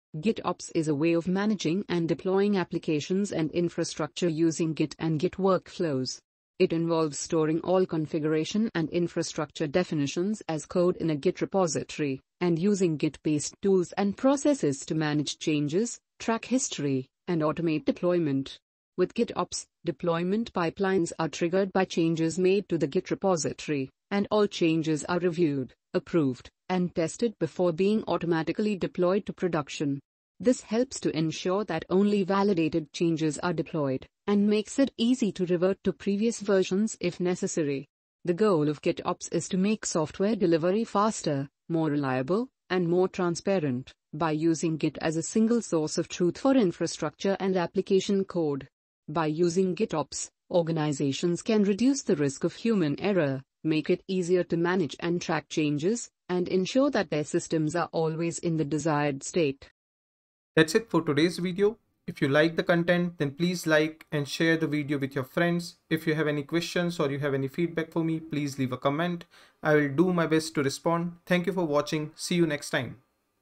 DevOps Interview Question 7: What is GitOps and How Does it Work in DevOps?
GitOps is a way of managing and deploying applications and infrastructure using Git and Git workflows.